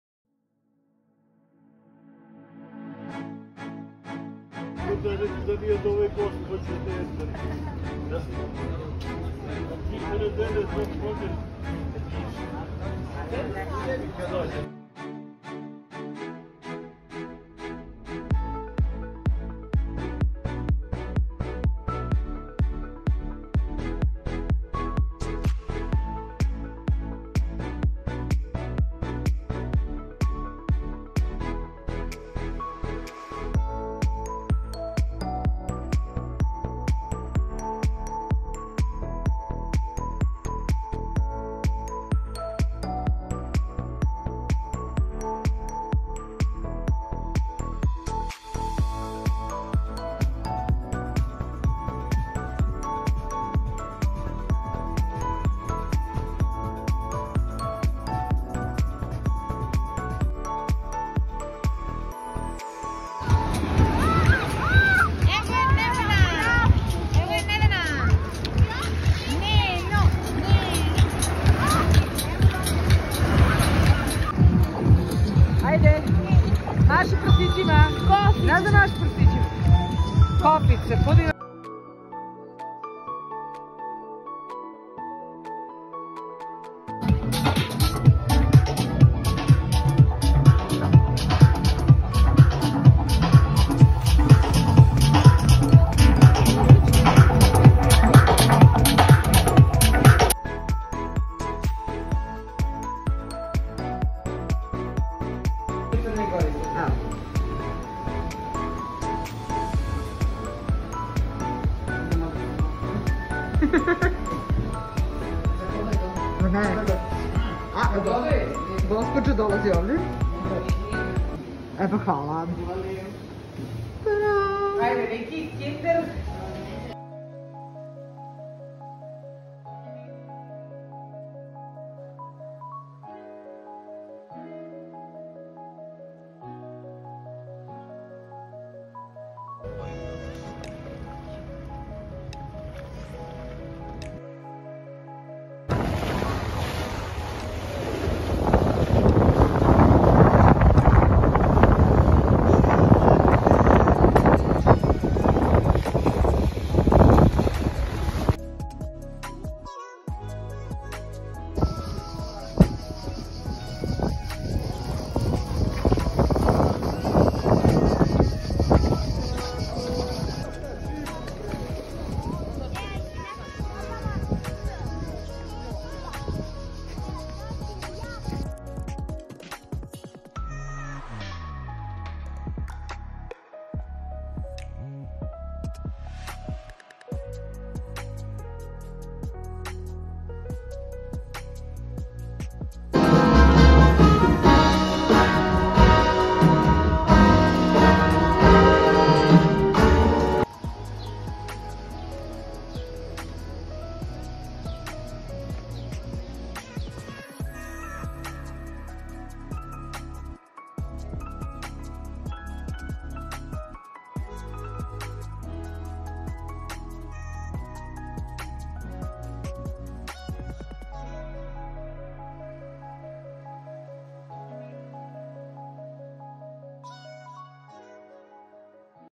Montenegro 2021
Short 4-day trip to Herceg Novi, Montenegro, July 2021.